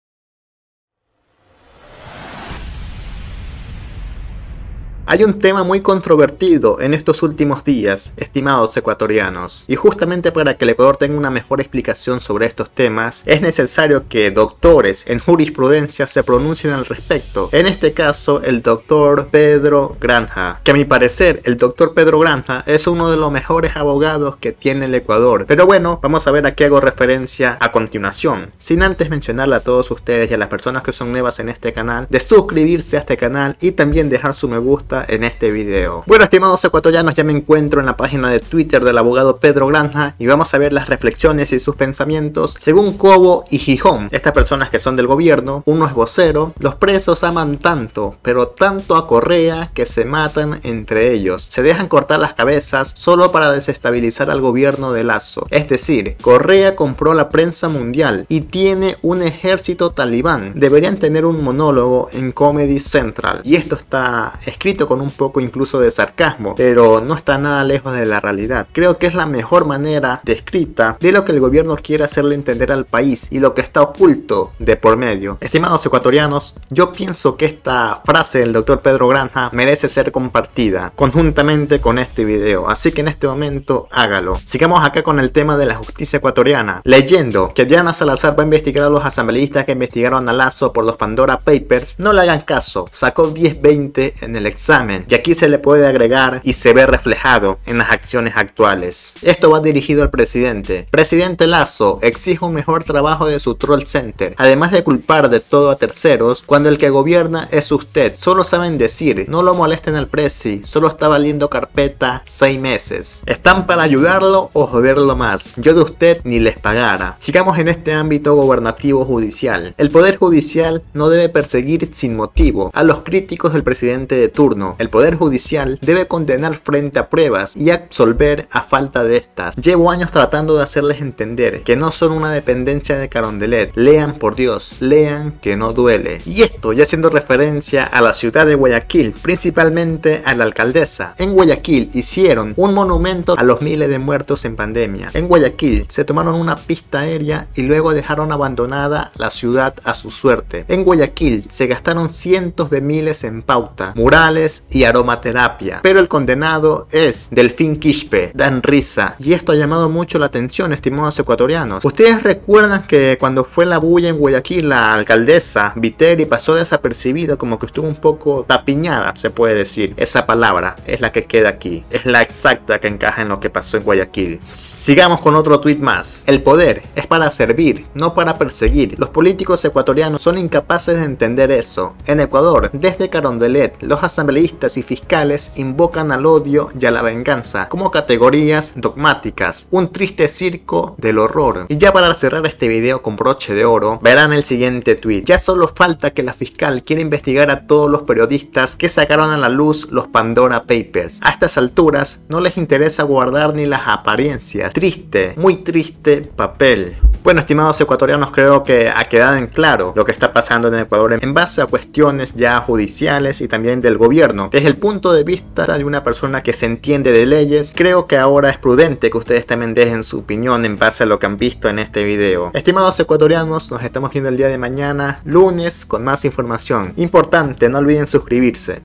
Pedro Granja le saca los cueros al sol a Lasso y Salazar
Bienvenidos al canal de noticias amateur de Ecuador:
Escritor Redactor y Creador de Novedades y Noticias de Último momento.